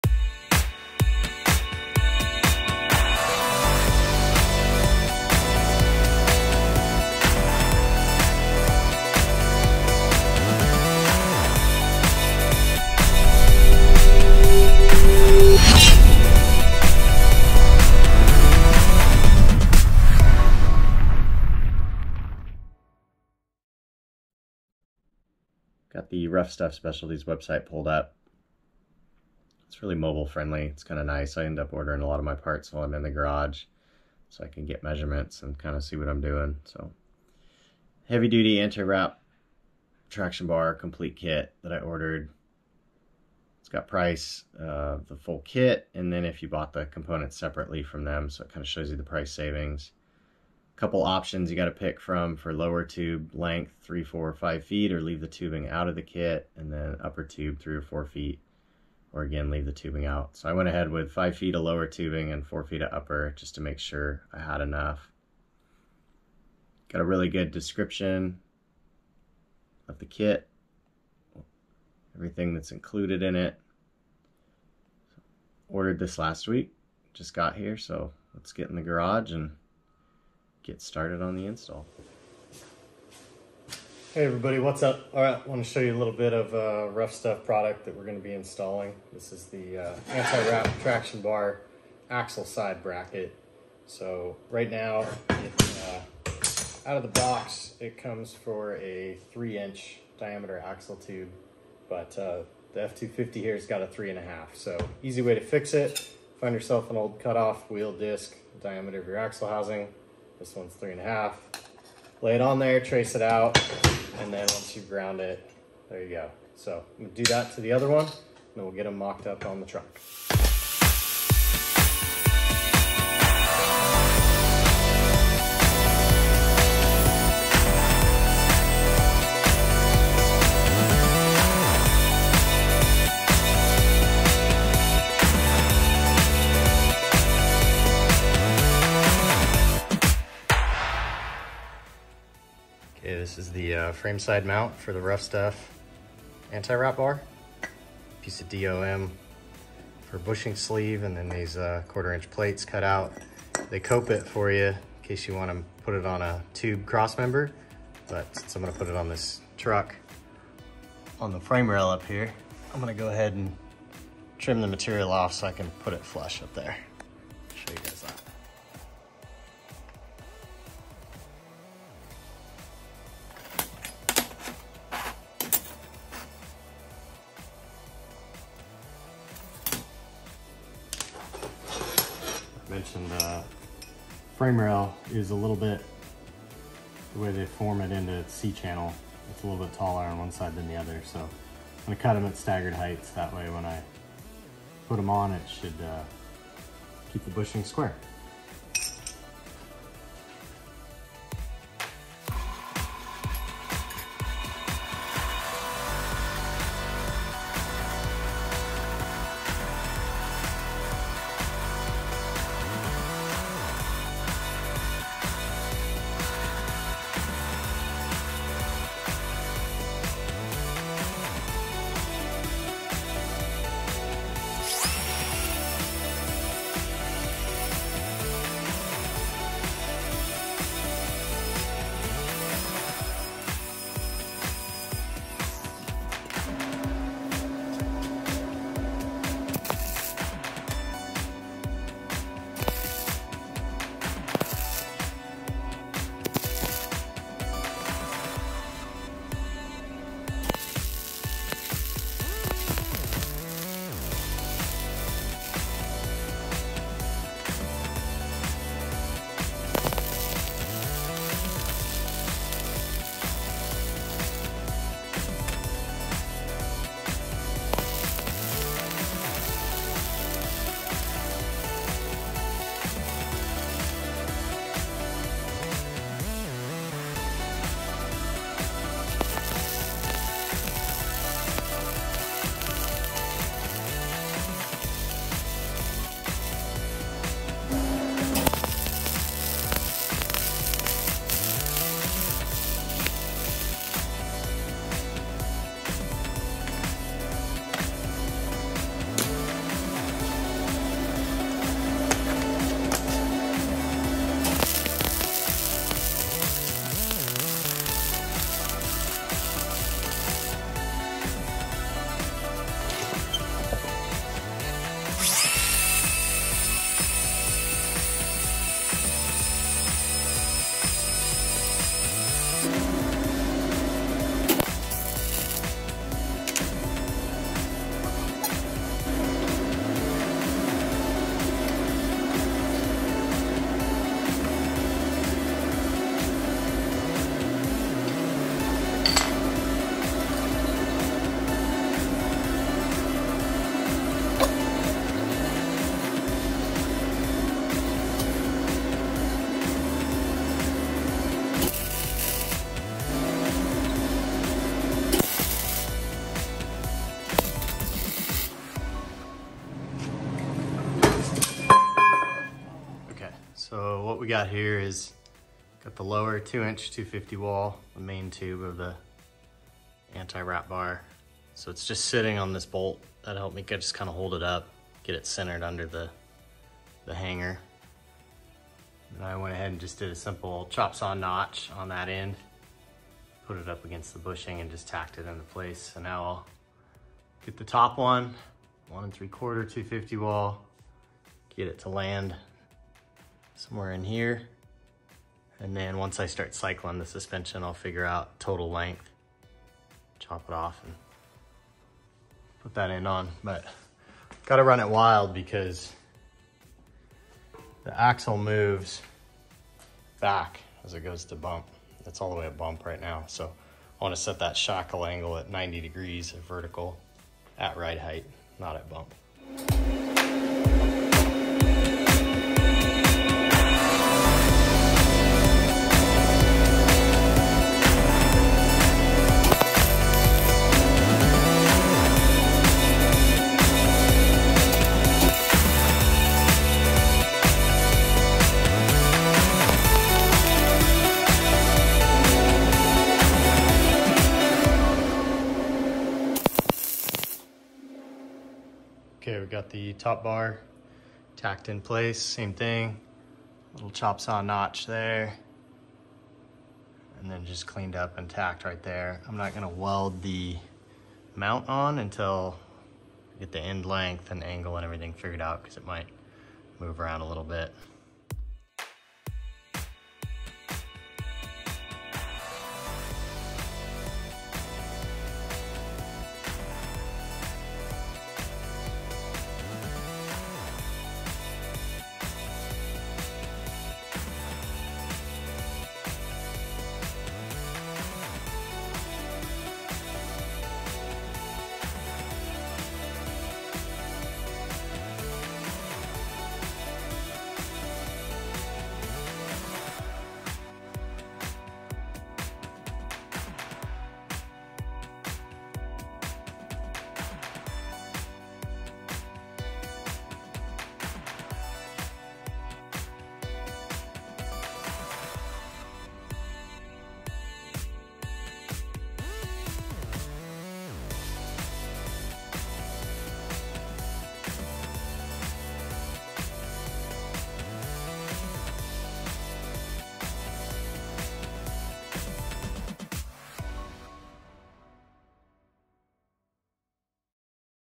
Ruffstuff Specialties Antiwrap Install, part 1 of 2
While building the long travel leaf spring suspension for my F250, I was worried about rear axle wrap with the soft leaf springs.  I ordered this kit from RuffStuff Specialties and decided to document the install after not finding much about antiwrap setup on the internet.